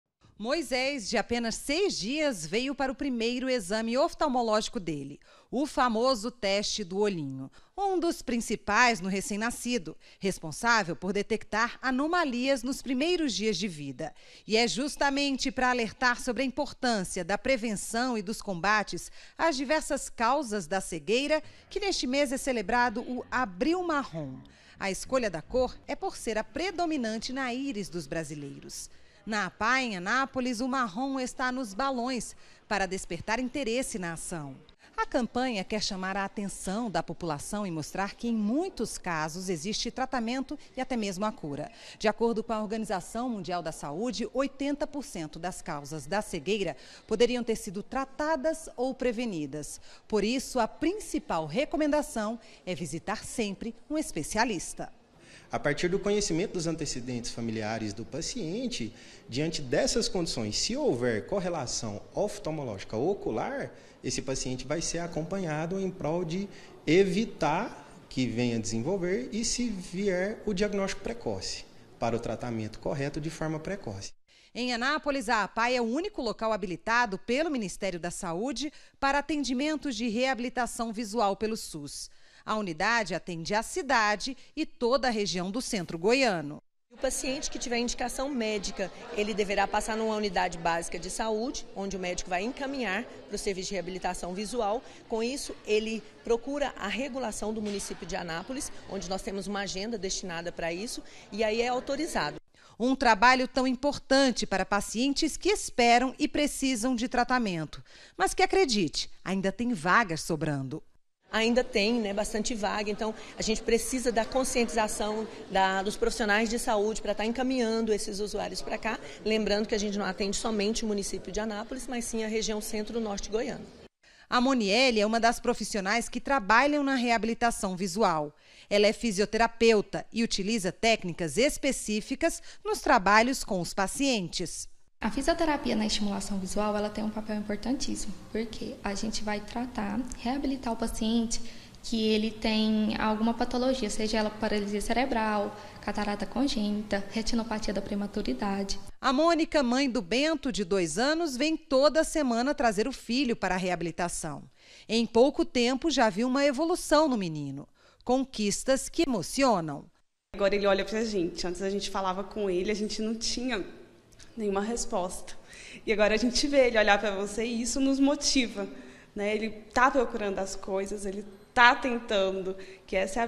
PREVENÇÃO E COMBATE À CEGUEIRA COMEÇA DESDE CEDO
ABRIL É O MÊS ESCOLHIDO PARA A PREVENÇÃO E COMBATE À CEGUEIRA. E OS CUIDADOS DEVEM COMEÇAR CEDO. EM ANÁPOLIS, UM PROJETO OFERECE TRATAMENTO DE GRAÇA  PARA CRIANÇAS QUE NASCEM COM PROBLEMAS DE VISÃO.